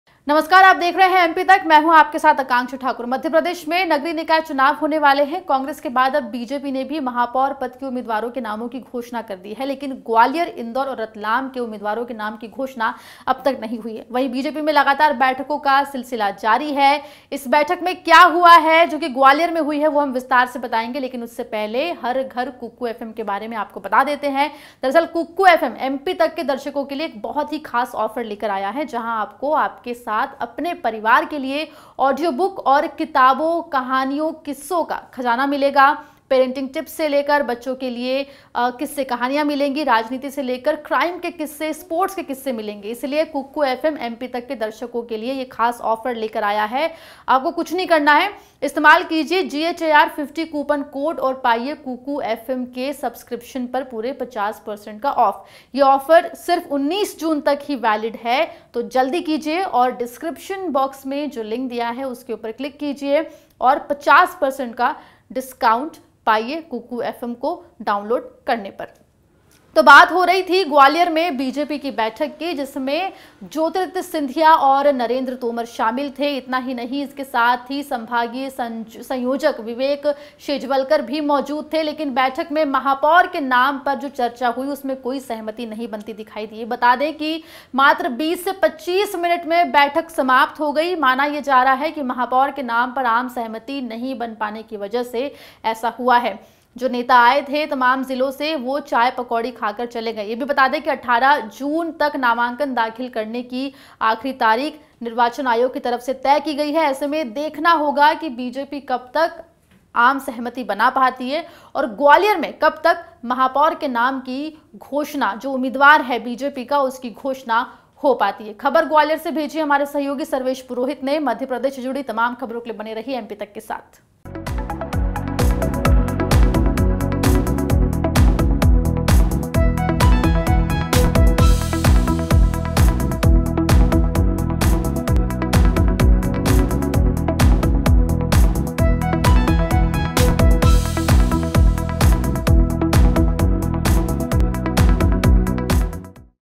ग्वालियर में बीजेपी का मेयर उम्मीदवार कौन होगा, क्या सिंधिया की चलेगी ? | MP Tak
ग्वालियर में बीजेपी का मेयर उम्मीदवार कौन होगा, क्या सिंधिया की चलेगी ? | MP Tak? देखिए पूरी रिपोर्ट..

Get Har Ghar KukuFM -